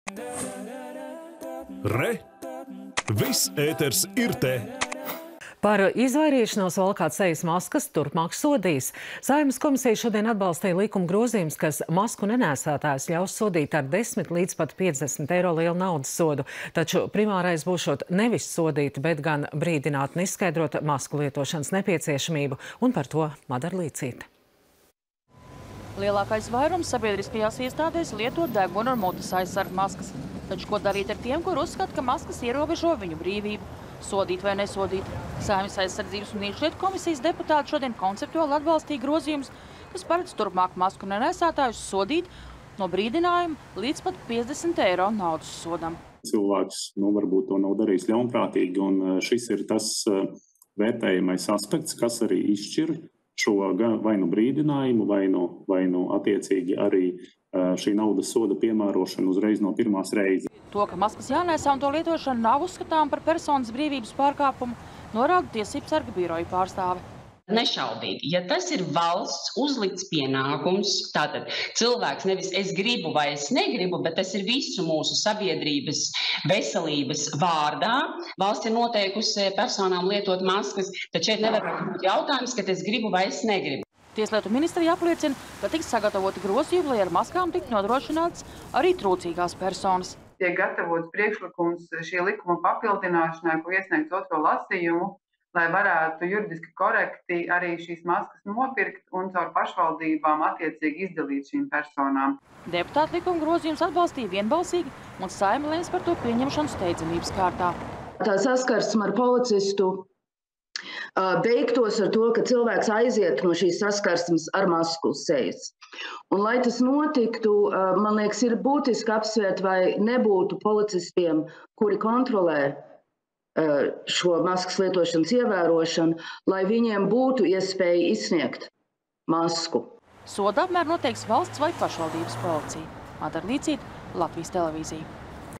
Sodīs masku nenēsātājus
Par izvairīšanos valkāt sejas maskas turpmāk sodīs. Saeimas komisija šodien atbalstīja likuma grozījumus, kas masku nenēsātājus ļaus sodīt ar 10 līdz pat 50 eiro lielu naudas sodu. Taču primārais būšot nevis sodīt, bet gan brīdināt  un izskaidrot masku lietošanas nepieciešamību. Turpina Madara Līcīte